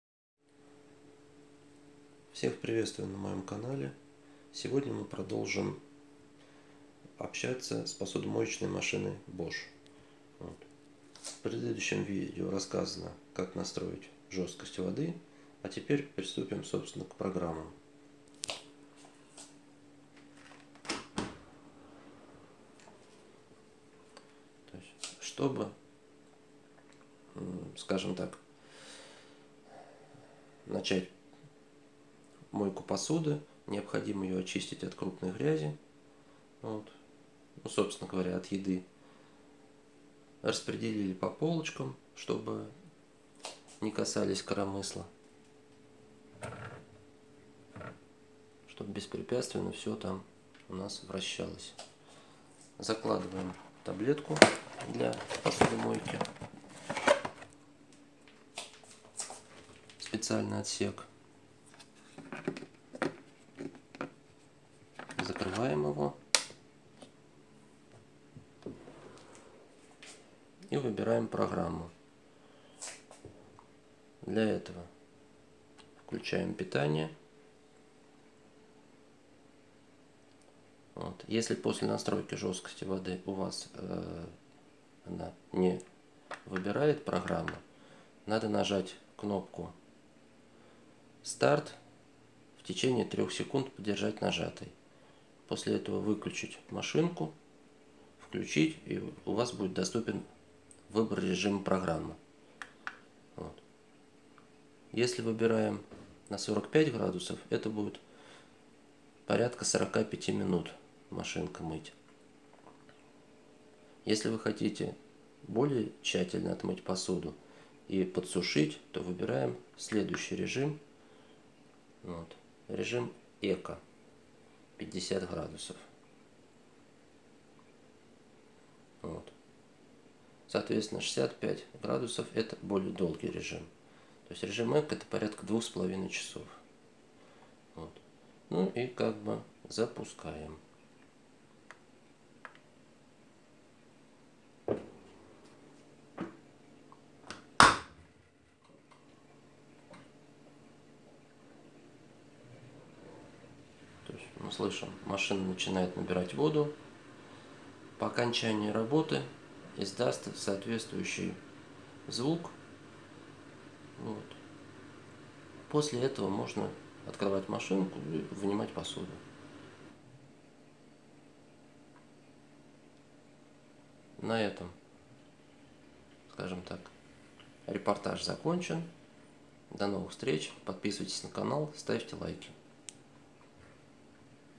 Посудомоечная машина bosch как правильно пользоваться, выбор программ.Как включить посудомойку bosch
Как пользоваться посудомоечной машиной Bosch выбор программ, как запустить посудомойку bosch, посудомоечная машина BOSCH SPV отзывы.
В этом видео рассказывается о запуске посудомоечной машины Bosch SPV40X80 с посудой и аналогичных моделей. О первом запуске можно посмотреть в предыдущем видео, ссылка в конце описания.
Обо всем это вы узнаете посмотрев это видео, а также предыдущее. В качестве моющего средства перешел на использование таблеток BioMio, отмывает хорошо.
Если нужна еще какая-то информация о данной машине, пишите в комментариях, с удовольствием отвечу на ваши вопросы.